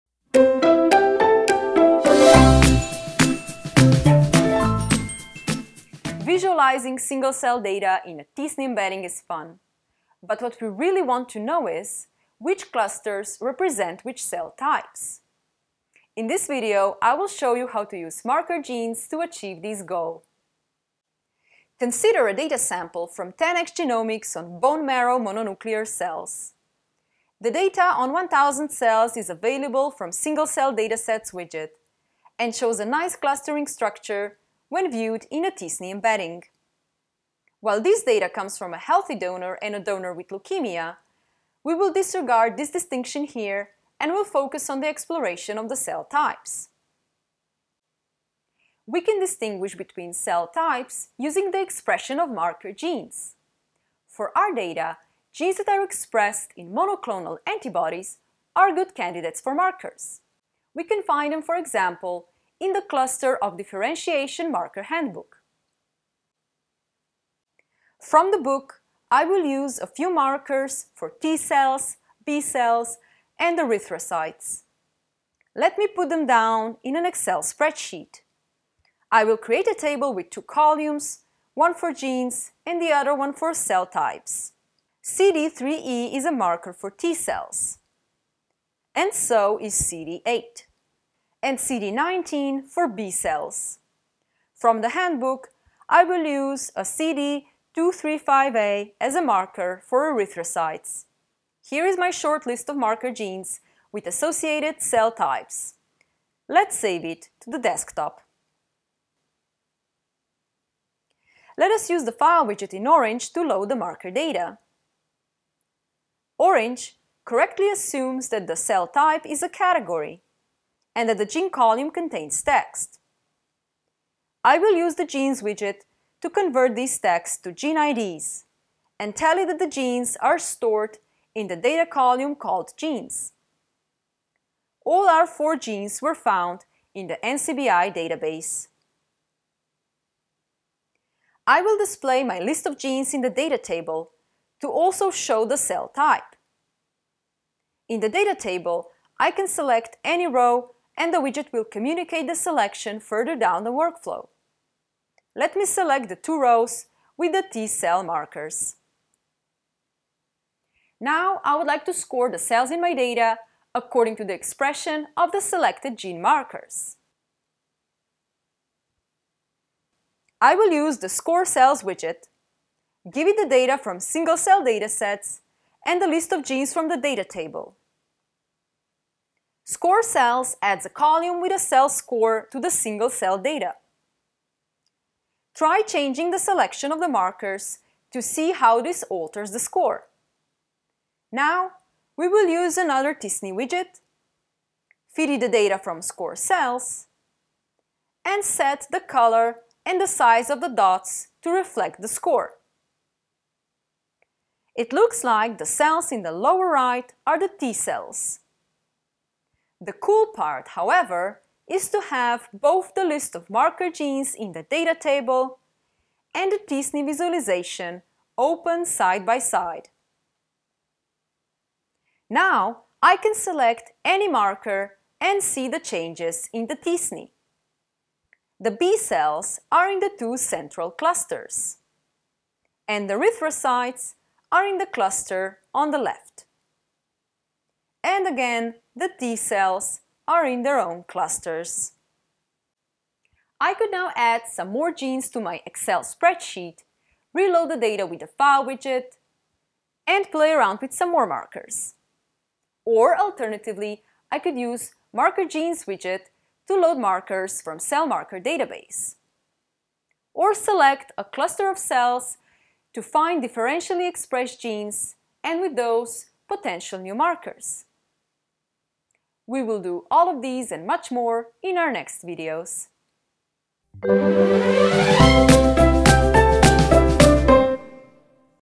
Single Cell Orange 05: Scoring Cells by Marker Genes
How to identify which clusters represent which cells by using marker genes and intelligent visualizations.

Design: Agnieszka Rovšnik
Created by: Laboratory for Bioinformatics, Faculty of Computer and Information Science, University of Ljubljana